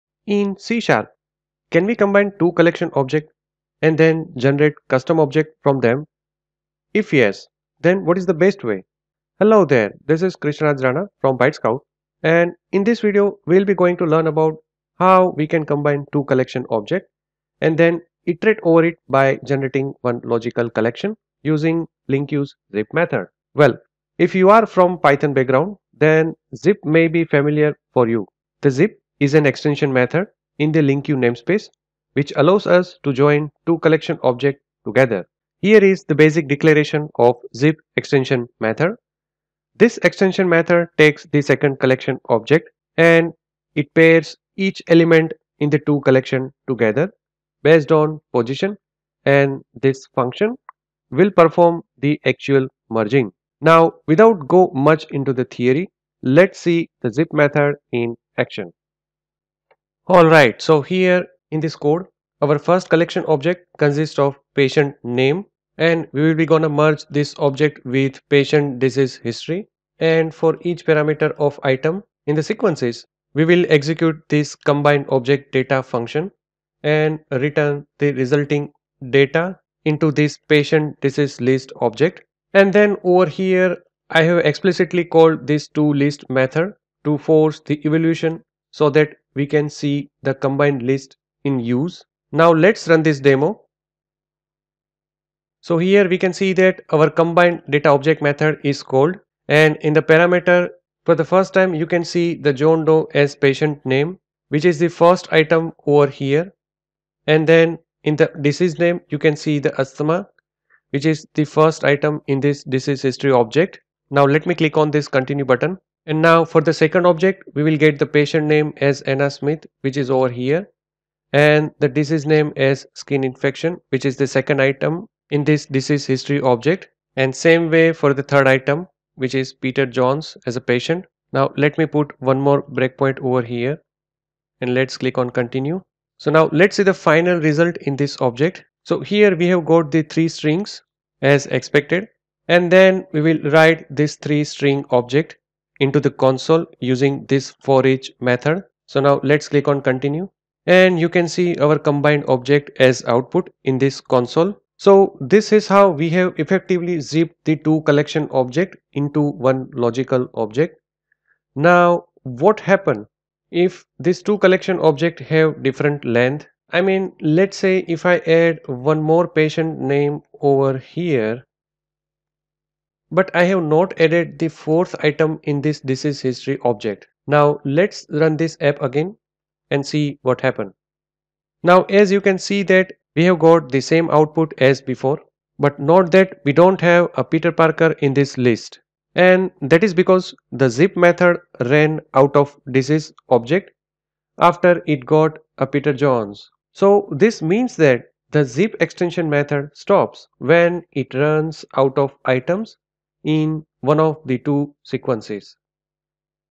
How to Merge 2 Collection Objects using LINQ's Zip Method - Part 1
The Zip is an extension method that merges the corresponding elements of two collection objects using a specified selector function. In this video, we'll be going to learn about how we can combine two collection objects and then iterate over them by generating one logical collection using LINQ's zip extension method.